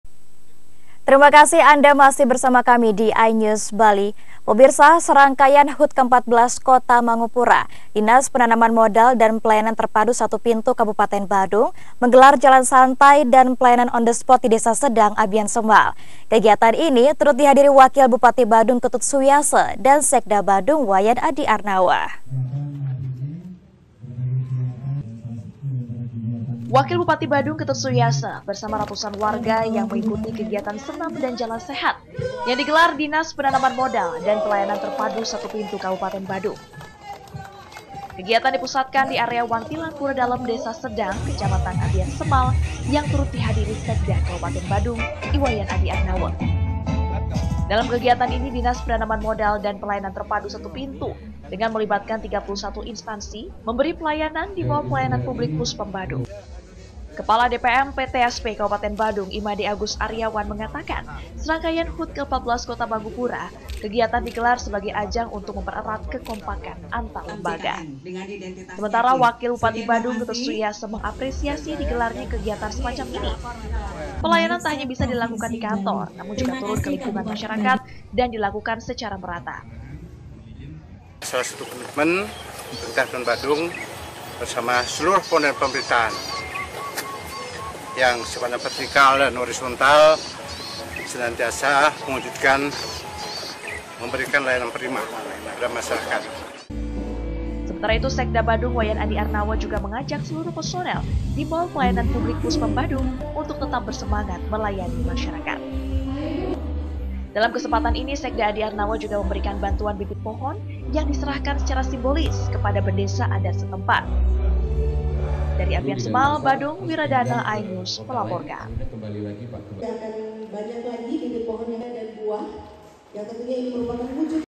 Wakil Bupati Badung Ketut Suiasa Mengapresiasi Pelayanan On The Spot Di Desa
Abiansemal, Badung - iNews Bali, 14 November 2023, Wakil Bupati Badung Ketut Suiasa Mengapresiasi Pelayanan On The Spot Di Desa